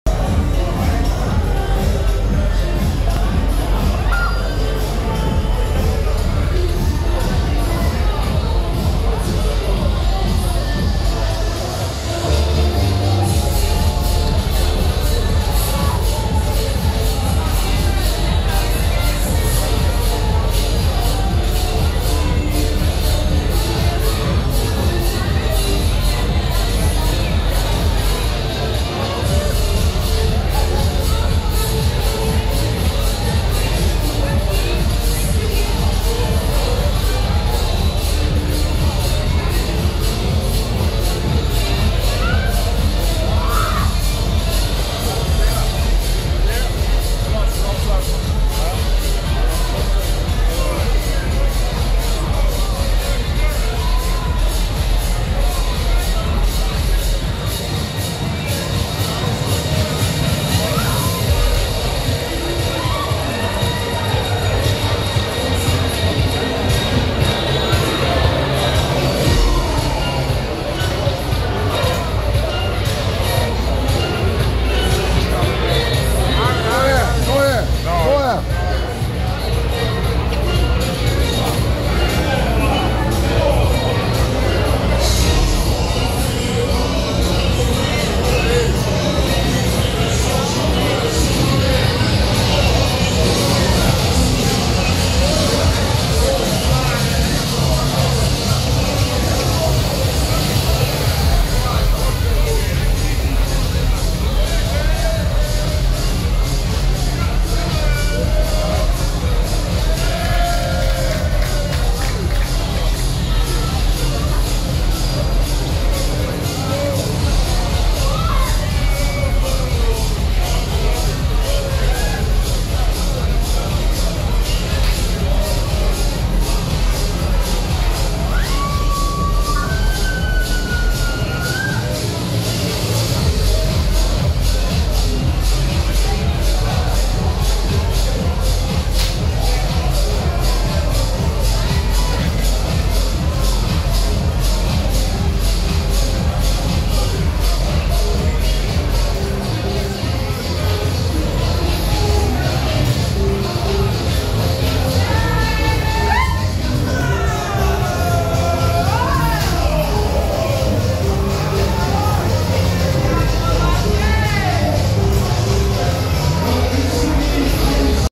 Spanish Resort Rodeo😮 Pretty Girls Riding on a Bull in Benidorm | Mechanical Bull 4K. EPIC FAIL
Recommended to watch at a reduced speed of 0.25x 😉